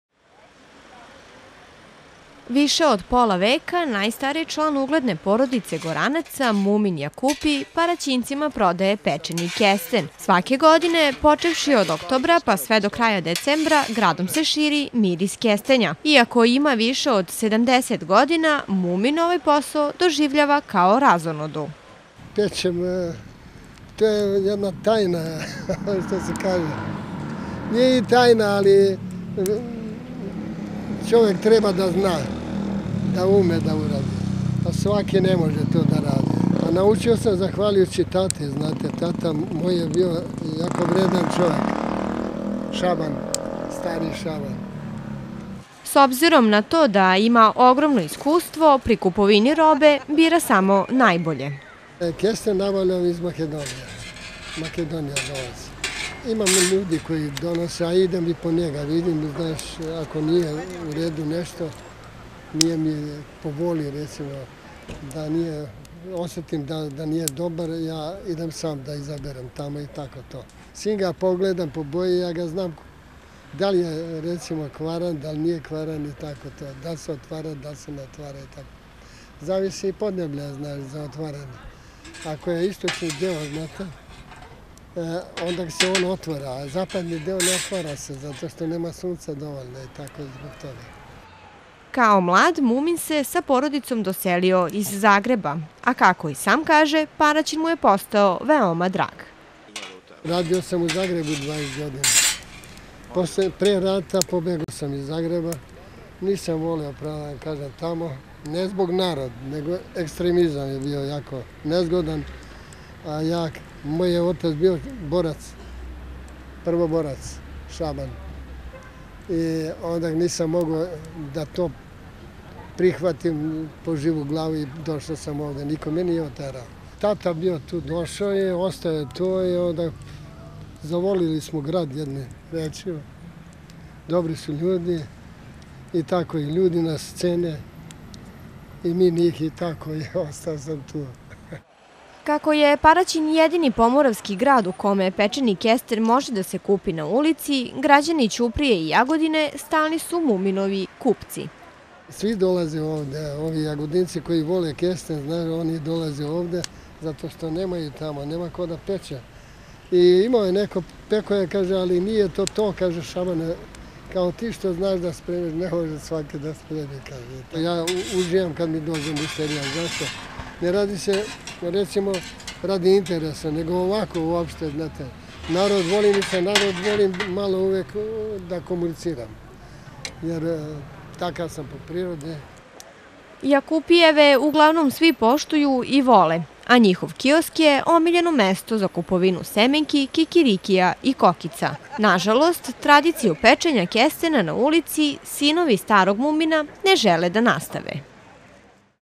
Pečen kesten
Svake godine, počevši od oktobra pa sve do kraja decembra, Paraćinom se širi miris pečenog kestena. Iako ima više od 70 godina, Mumin Jakupi, I dalje peče I prodaje kesten u glavnoj gradskoj ulicu.